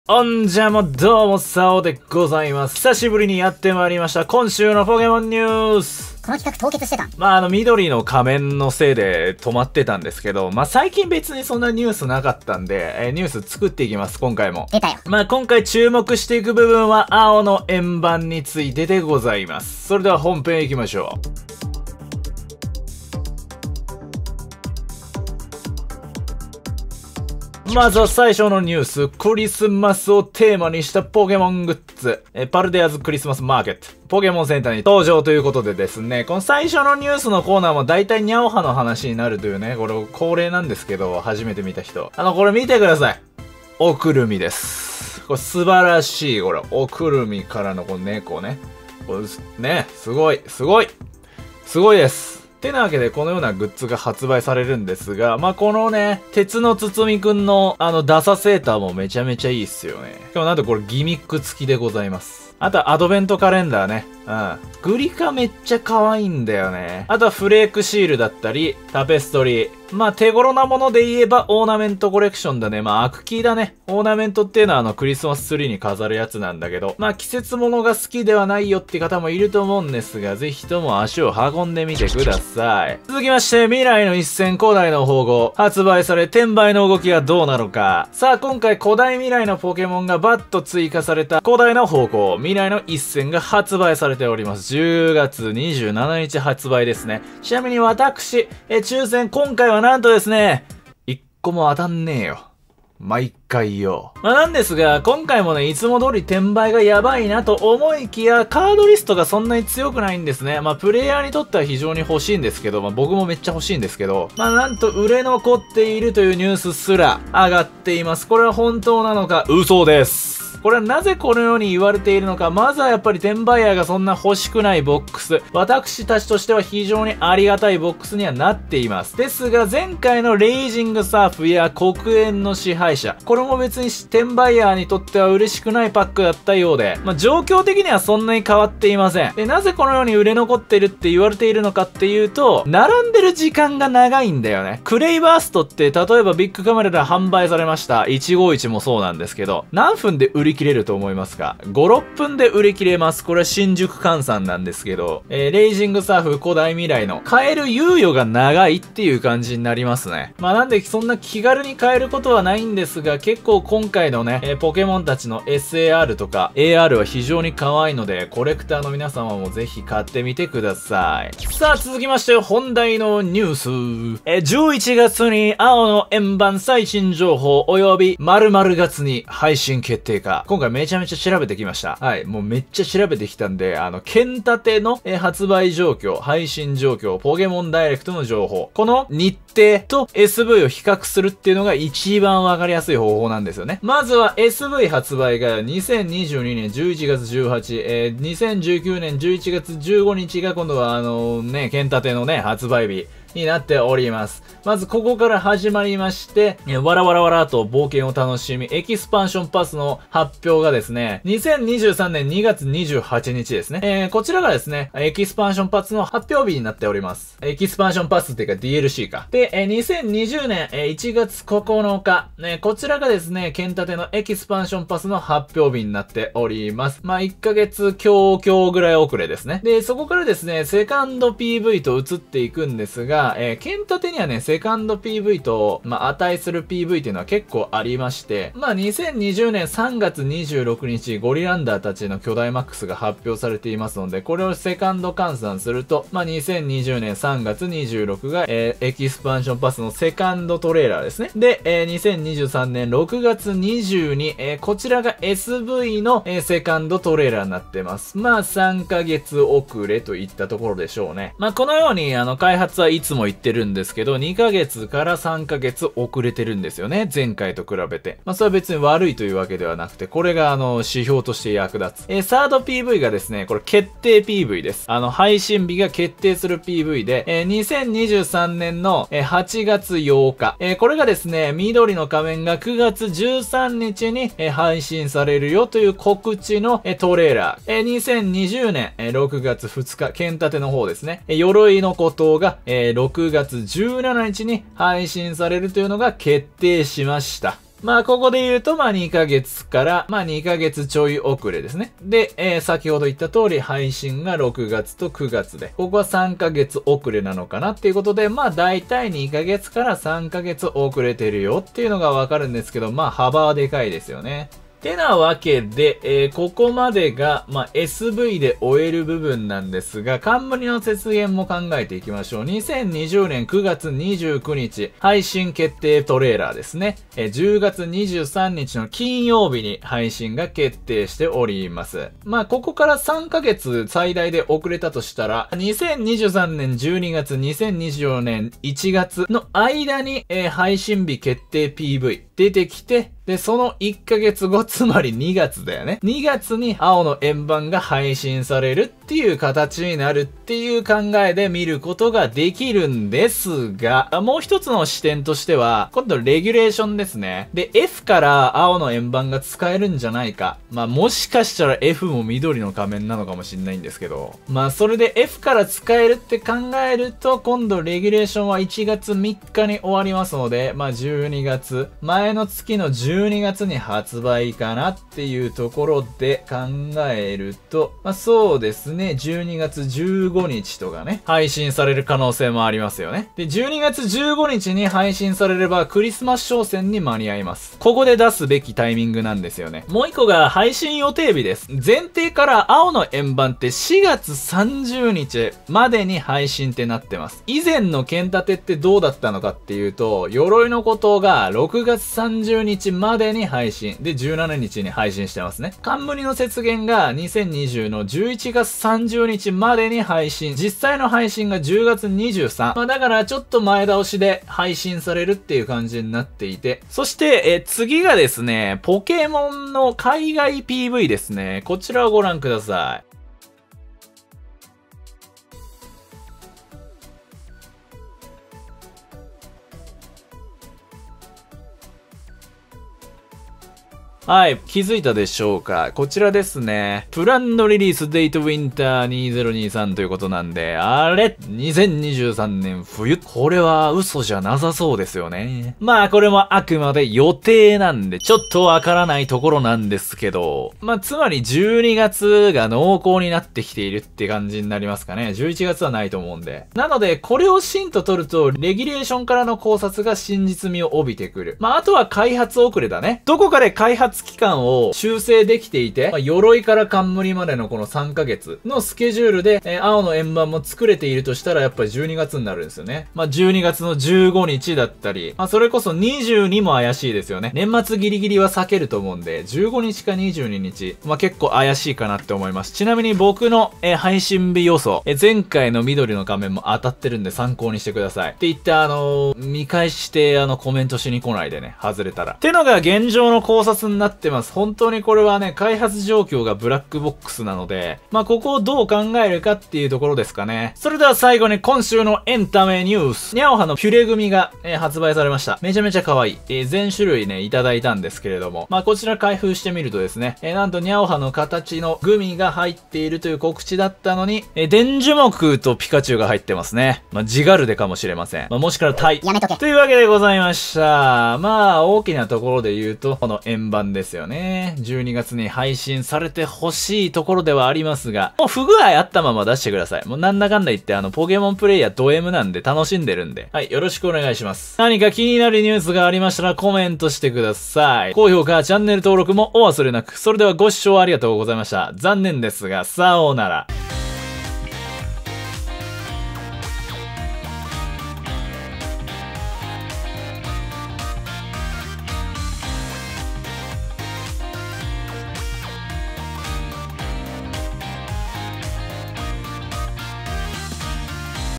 【結局いつ？】DLC”藍の円盤”配信開始日を深掘り予想・考察！新情報公開まもなくか！？/ポケカ新弾売れ残りがやばい...【今週のポケモンニュース】【碧の仮面/藍の円盤】
帰ってきた今週のポケモンニュース！！ポケカから藍の円盤まで！！色々取り扱ってます！今回のメイントピックは藍の円盤の配信日についてです！

新プレゼント送り先
〒106-0031
東京都港区西麻布2-13-12 早野ビル604 株式会社ライバー さおう宛